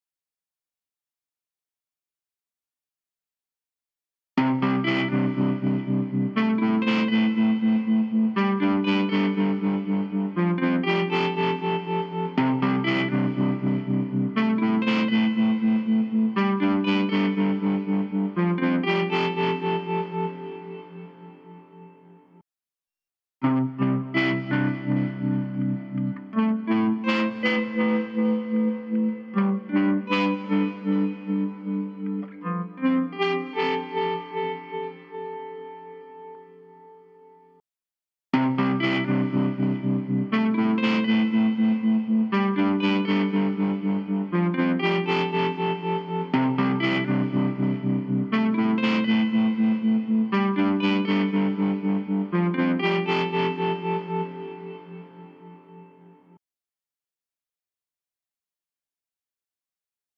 Guitar Lesson: How to copy the sound of a tremolo pedal
Bring The Noise: Coax crazy sounds from your axe: #61 Tremo-faux

Download Total Guitar issue 262 for the accompanying explanation - available in the UK and, digitally, worldwide via Newsstand for iPad, iPhone & iPod touch and Zinio from 22 December 2014.